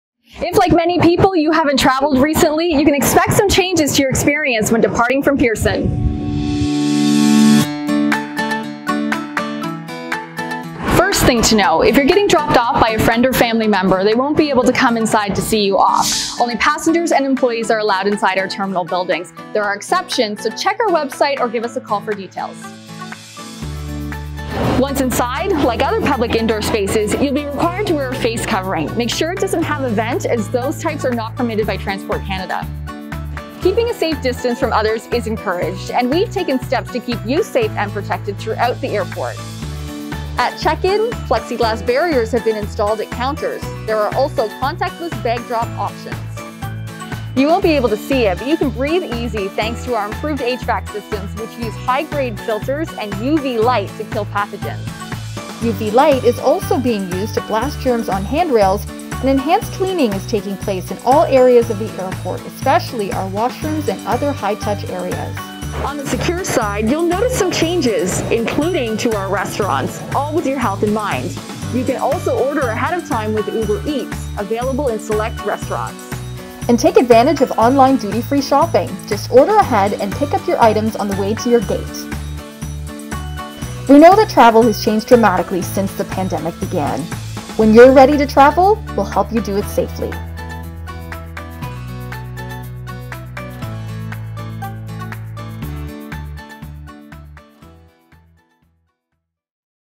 Ready To Travel - Departures
When you're ready to travel, we'll help you do it safely. In this video, you'll learn what to expect when flying from Toronto Pearson.